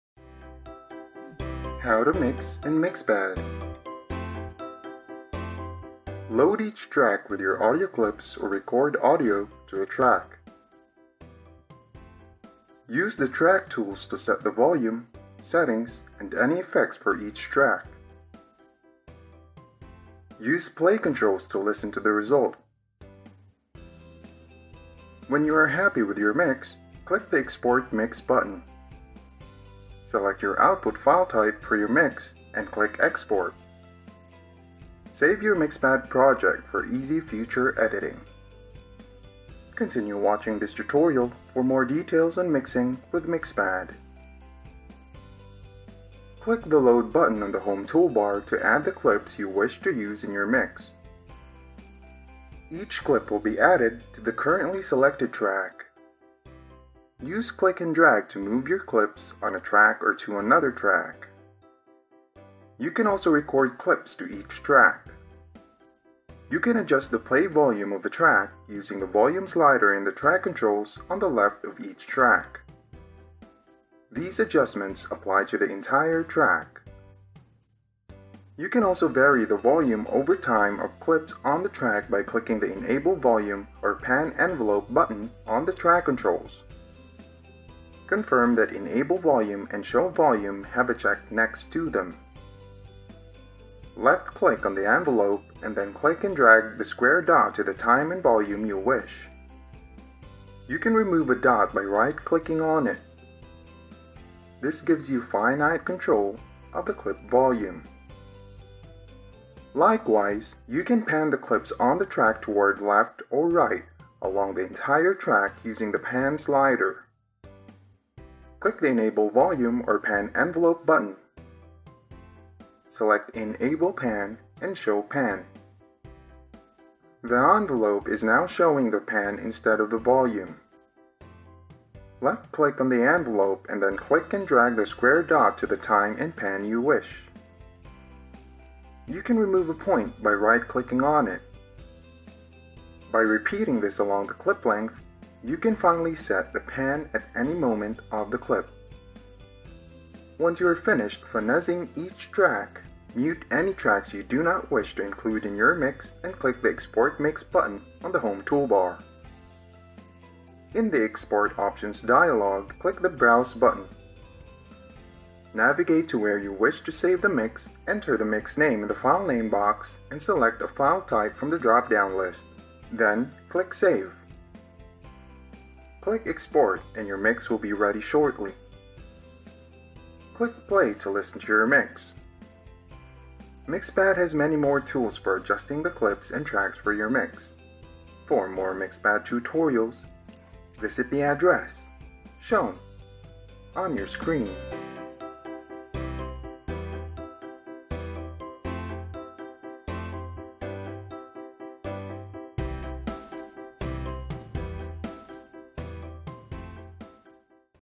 02 - MixPad - Come missare
Come missare in MixPad.
Carica ogni traccia con le tue clip audio. o registra audio su una traccia.
Usa gli Strumenti traccia per impostare il volume,
 le impostazioni ed eventuali effetti per ogni traccia.
Usa i controlli di riproduzione per ascoltare il risultato.
Quando sei felice con il tuo mix
fai clic sul Esporta mix.
Seleziona il tipo di file di output per il tuo mix e fai clic su Esporta.
Salva il tuo progetto MixPad per permettere facile modifica in futuro.
Continua a guardare questo tutorial per più dettagli sul missaggio con MixPad.
Fai clic sul pulsante Carica sulla barra degli strumenti Home
per aggiungere le clip che desideri utilizzare nel tuo mix.
Ogni clip verrà aggiunta alla traccia attualmente selezionata.
Usa clicca e trascina per spostare le clip su di una traccia o su un'altra traccia.
Puoi anche registrare clip su ogni traccia.
È possibile regolare il volume di riproduzione della traccia
utilizzando il cursore Volume nei controlli traccia a sinistra di ciascuna traccia.
Queste regolazioni vengono applicate all'intera traccia.
Puoi anche variare il volume delle clip nel tempo sulla traccia
facendo clic sui pulsanti Abilita volume oppure Panoramica inviluppo
sui controlli della traccia.
Conferma che Abilita volume e Mostra
volume abbiano un visto (V) accanto ad essi.
Fai clic sull'Inviluppo,
quindi fai clic e trascina il punto quadrato all'ora e al volume che desideri.
Puoi rimuovere un punto facendo clic destro su di esso.
Questo ti dà il controllo finito del volume della clip.
Allo stesso modo puoi dare Panoramica alle clip sulla traccia verso sinistra o verso destra
lungo tutta l'intera traccia utilizzando il cursore della panoramica.
Fai clic sul pulsante Abilita volume o
Panoramica inviluppo.
Seleziona Abilita panoramica e Mostra panoramica.
L'inviluppo ora mostra la panoramica invece del volume.
Fai clic sinistro sull'inviluppo
e poi fai clic e trascina il punto quadrato sull'ora e la panoramica  che desideri.
Puoi rimuovere un punto facendo clic con il pulsante destro del mouse su di esso.
Ripetendo questo lungo la lunghezza della clip c
puoi finalmente impostare la panoramica in qualsiasi momento della clip.
Una volta che hai finito di completare il processo su ciascuna traccia,
silenzia tutte le tracce che non desideri includere nel tuo mix
e fai clic sul pulsante Esporta mix sulla barra degli strumenti principale.
Nel dialogo delle Opzioni di esportazione fai clic sul pulsante Sfoglia,
vai nella posizione in cui desideri salvare il mix,
inserisci il nome del mix nella casella del nome del file e seleziona il tipo di file
dall'elenco a discesa, quindi fai clic su Salva.
Fai clic su Esporta e il tuo mix sarà pronto a breve.
Fai clic su Riproduci per ascoltare il tuo mix.
MixPad ha molti altri strumenti per regolare le clip e le tracce per il tuo mix.
Per altri tutorial sul Mixpad
visita l'indirizzo
visualizzato
sul tuo schermo

Hai bisogno di aiuto con MixPad?